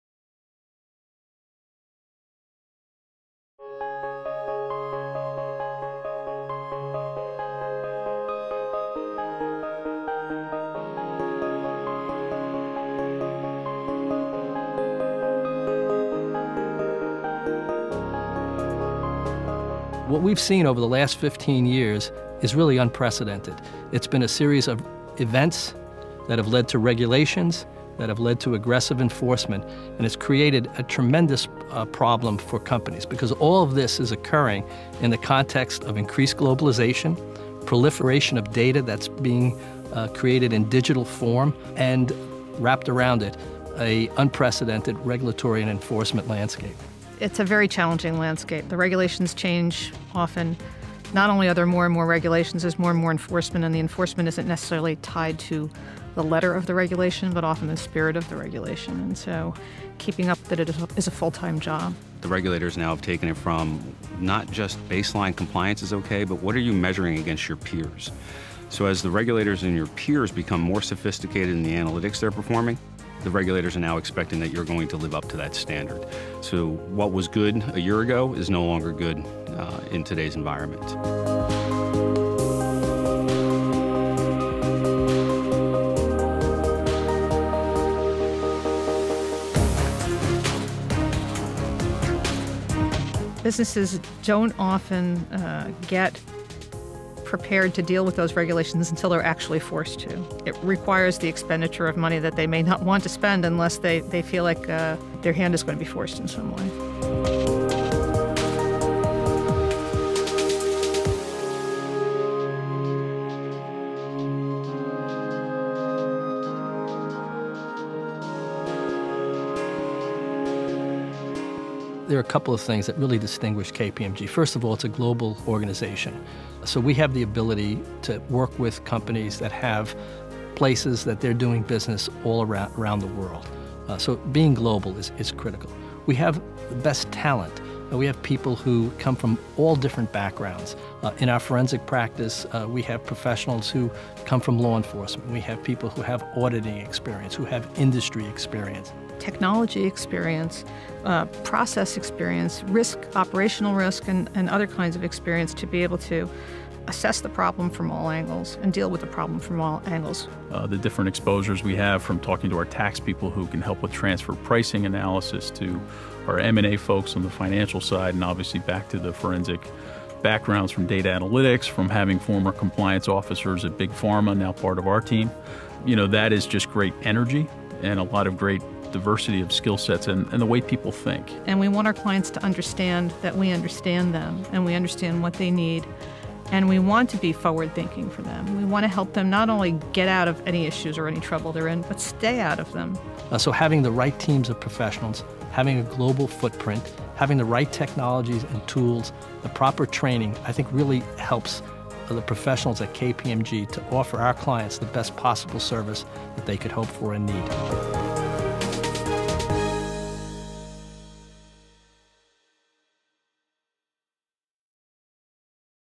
Managing Risk in the New Era of Regulatory Enforcement
KPMG Forensic professionals discuss the unprecedented changes in the regulatory and enforcement landscape over the last fifteen years.  They speak about the challenging and volatile environment that is creating a tremendous problem for business leaders and companies, as they try to keep up to effectively manage their risk and avoid increasing government fines and penalties. Hear KPMG’s Richard Girgenti, Teresa Pesce, Thomas Keegan and Marc Miller share their insights on this new era of regulatory enforcement.